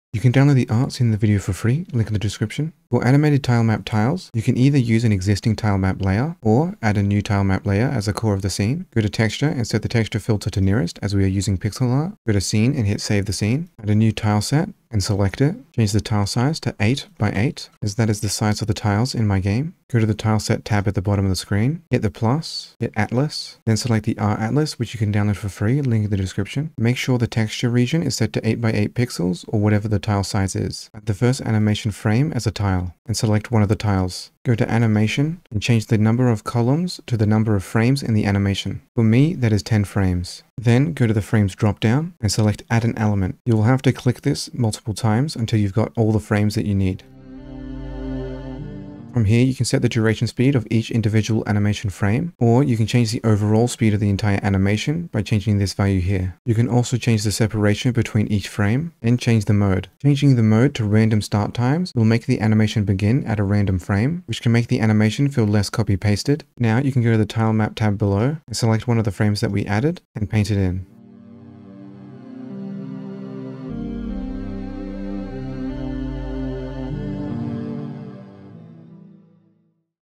Animate TileMap Tiles in Godot 4.4
Learn how to Animate TileMap Tiles in Godot 4.4

In this video you will learn about how to animate the individual tiles inside of a TileMap in the Godot Game Engine Version 4.4

As Thanks, Enjoy this Steam Key for Vylan!
72T0N-XHDM6-NI8PR